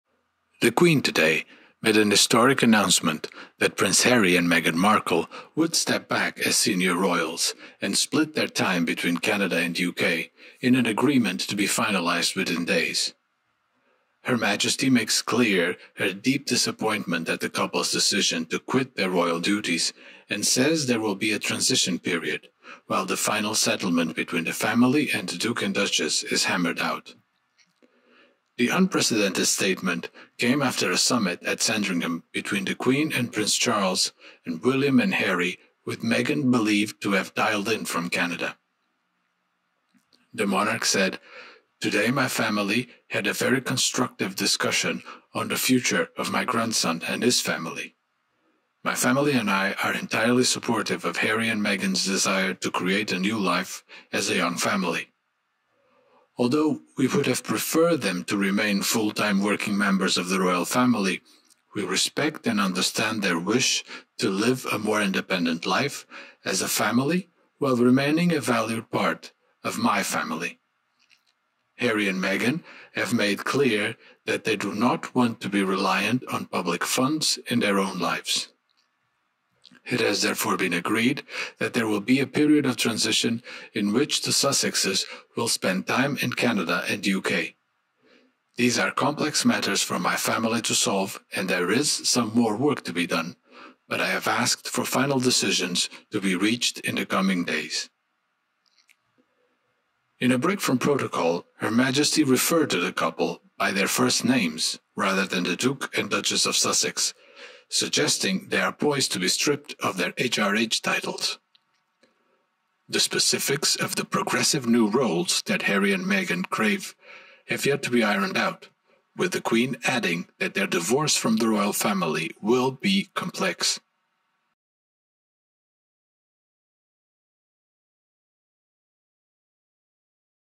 The Queen GIVES IN to Harry and Meghan and lets them quit
The Queen GIVES IN to Harry and Meghan and lets them quit: Her Majesty says she wanted couple to stay as 'full-time royals' - but agrees they can 'create a new life' in UK and Canada as part of 'transition period' to be finalised in DAYS